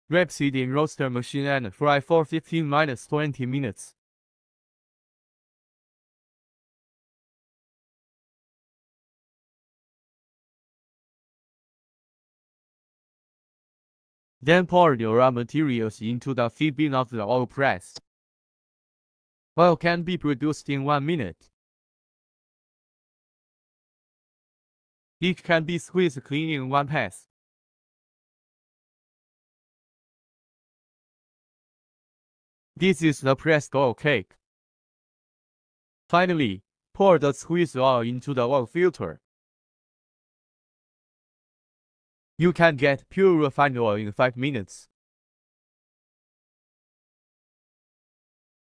The whole process of squeezing rapeseed by screw press
This oil press can be cold-pressed or hot-pressed. It can squeeze peanuts, soybeans, rapeseeds and other raw materials. It has a long service life and can be squeezed in one pass.